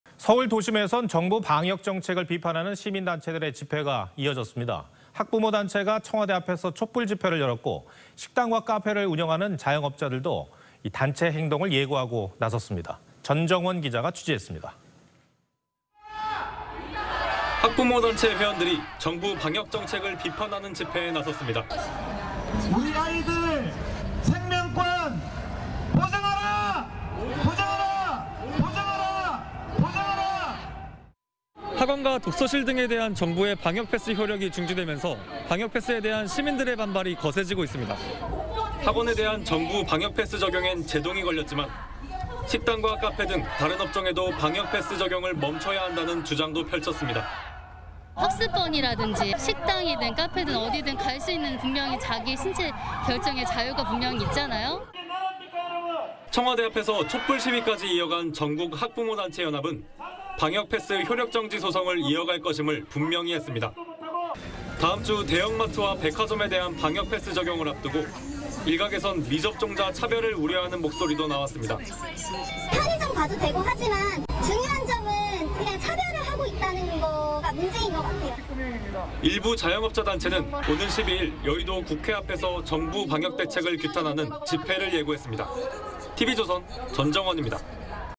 '방역패스 효력정지' 후폭풍…시민단체, 청와대 앞 촛불집회
서울 도심에선 정부 방역정책을 비판하는 시민단체들의 집회가 이어졌습니다. 학부모단체와 백신접종 피해자 단체가 청와대 앞에서 촛불집회를 열었고, 식당과 카페를 운영하는 자영업자도 단체행동을 예고하고 나섰습니다.